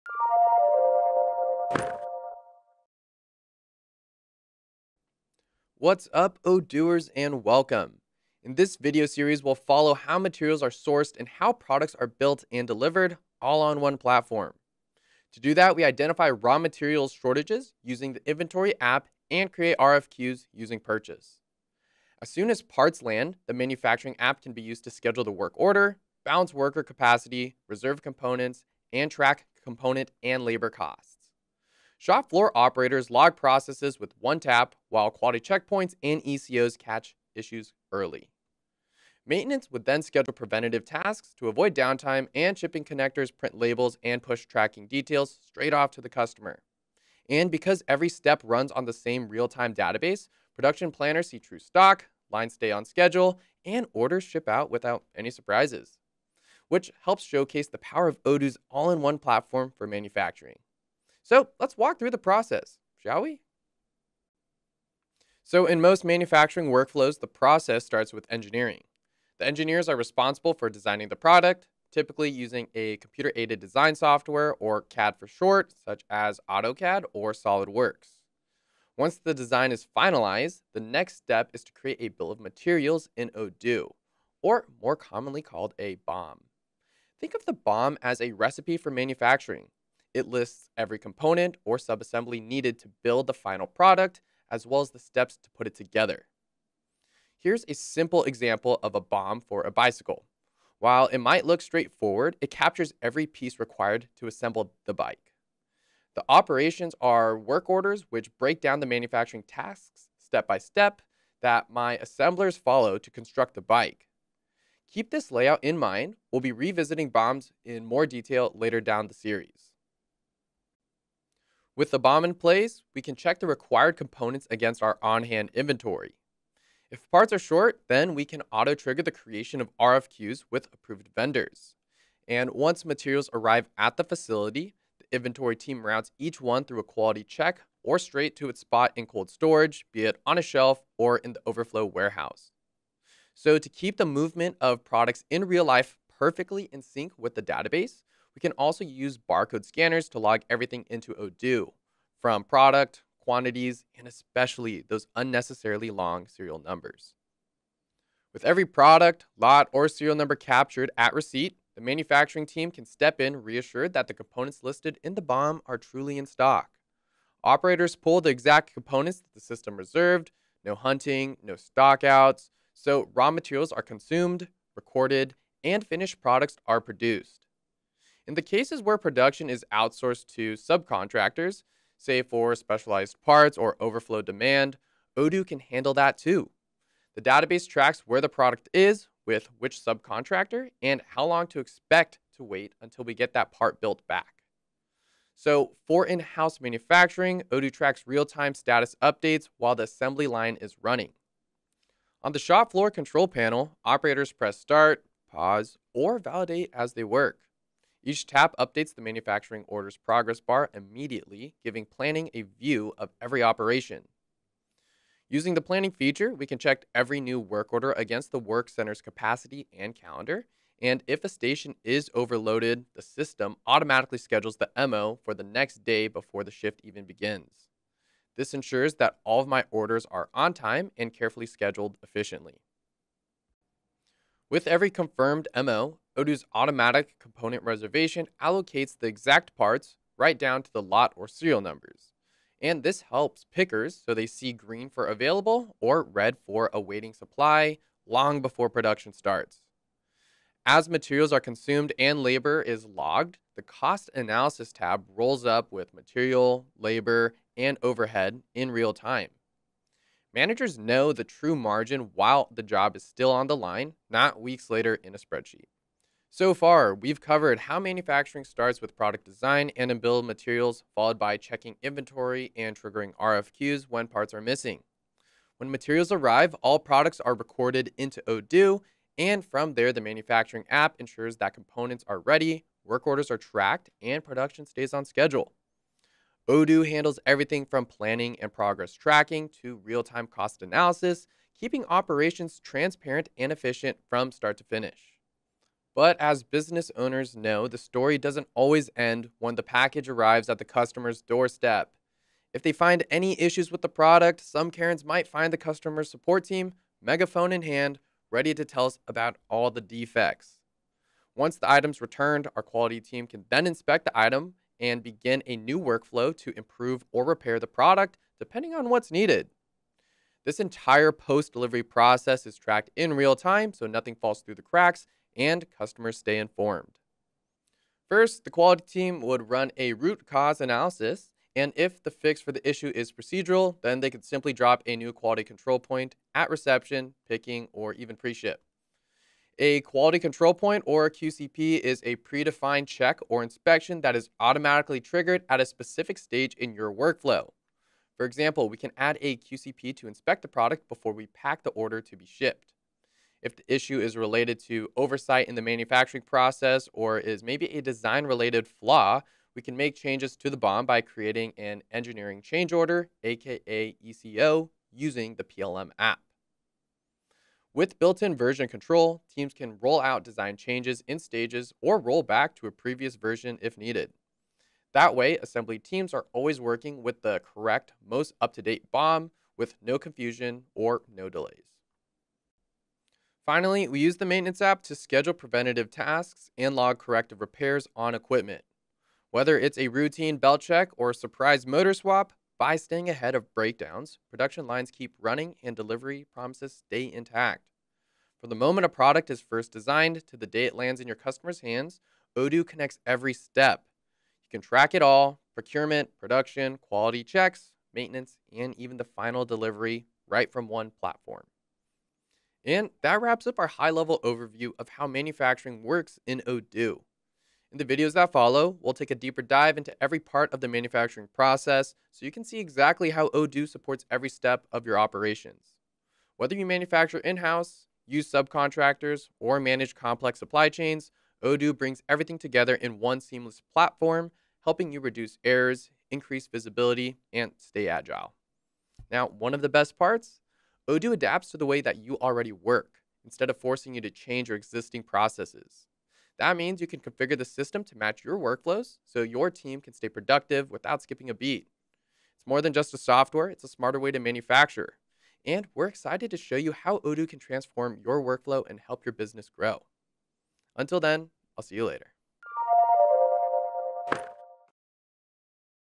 MRP Overview | Odoo MRP
In this video, learn how to to steamline production, manage work orders, and optimize your manufacturing operation from start to finish with Odoo MRP.

**App documentation**